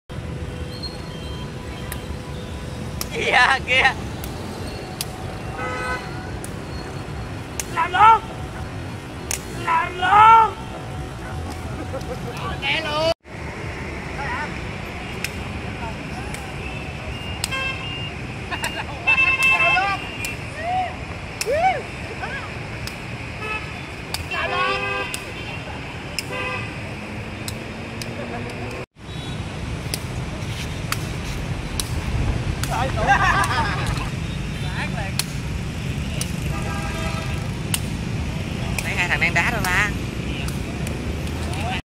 Đá Cầu Kiểng Đâm - Bình Bò mấy năm không đá
Đá Cầu Kiểng Đâm là một môn thể thao tự do, khác với đá cầu bình thường, đá cầu kiểng đâm chỉ yêu cầu 1 nhịp, không được đỡ cầu mà phải nhanh chóng đưa cầu sang đối phương, 1 cú đá nhanh như điện xẹt trúng người sẽ ghi được nhiều điểm.